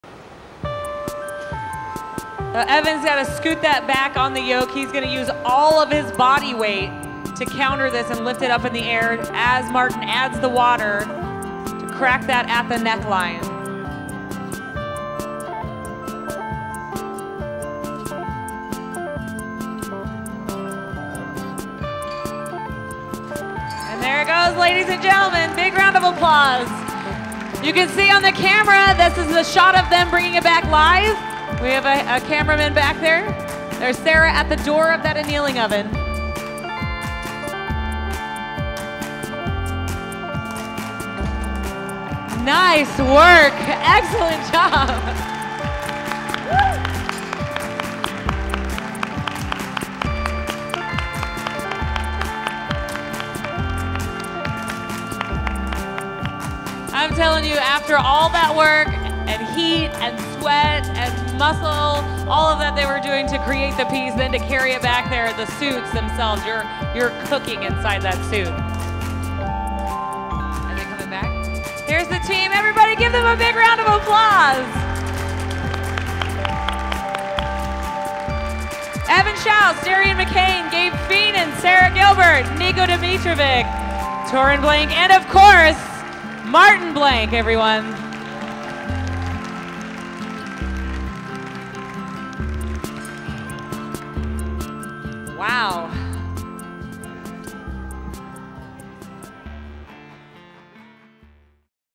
Martin Blank - Hot Shop Live
A sneak peak at the new online stream broadcast format "Hot Shop Live."